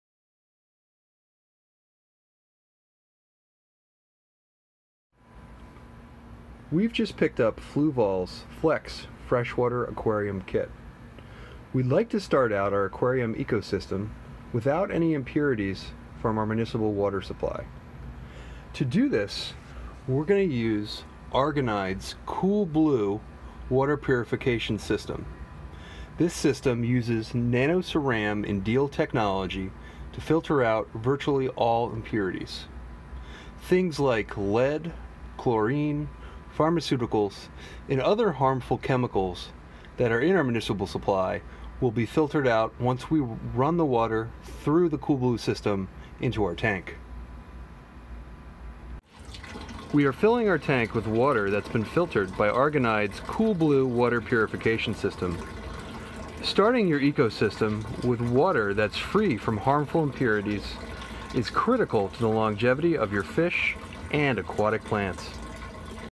Filling a Fluval Flex Using Argonide CoolBlue
Starting an aquarium echosystem with no lead, chlorine, pharmaceuticals or harmful chemicals using a home water filter.